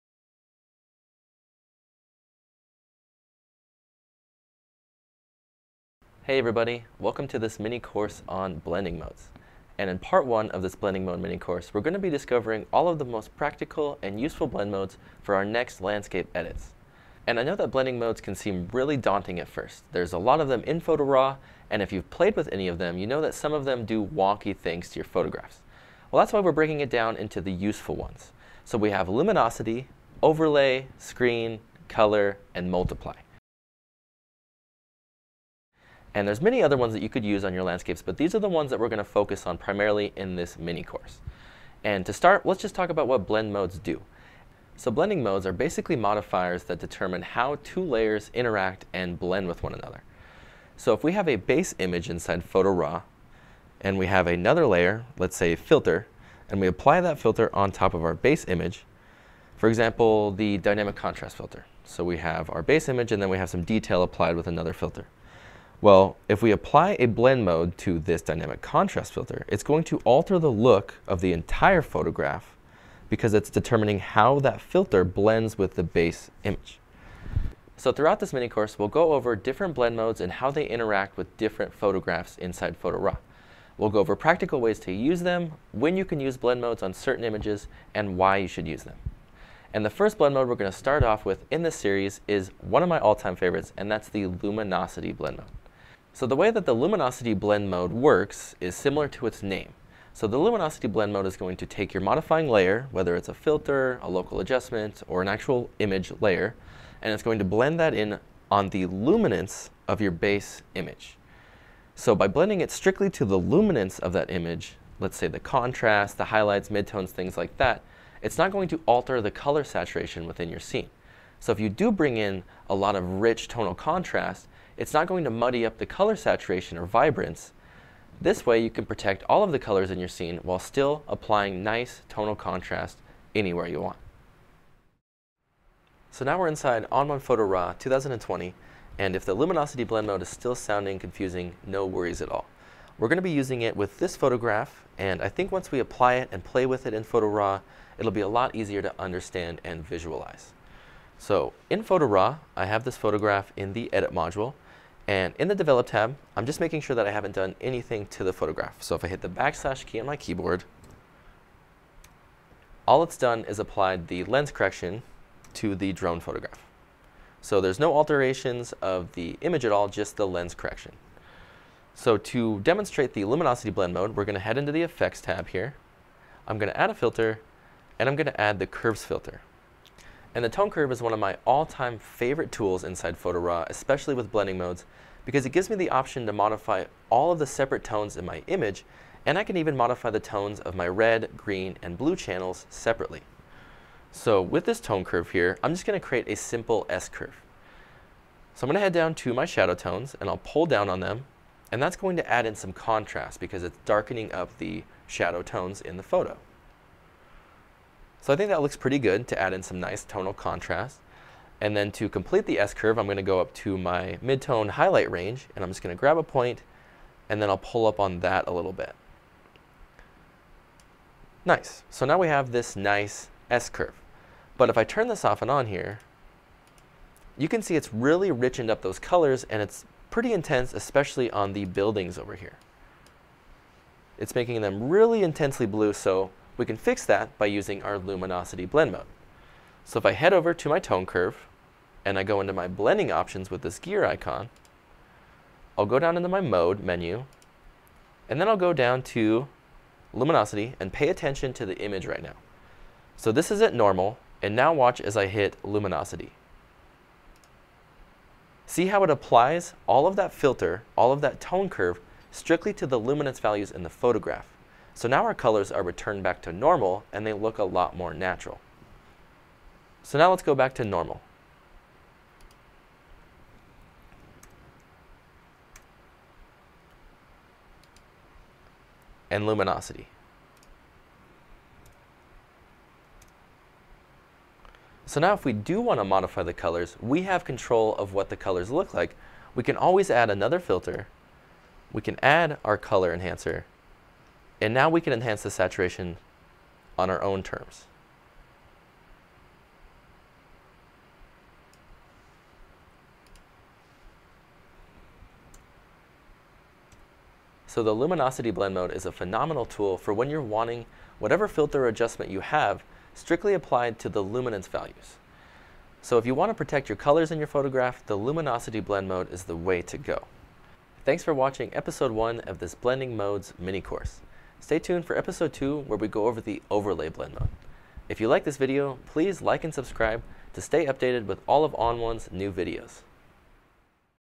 Blend Mode Mini-Course Part 1 | Episode 1: Luminosity
Watch the first episode of the all-new BlendingModes Mini-Course Part 1: Landscapes where we dive into the ever-helpful Luminosity Blend Mode. Great for protecting your colors when modifying contrast, see why this is one of the most useful blend modes in LandscapePhotography.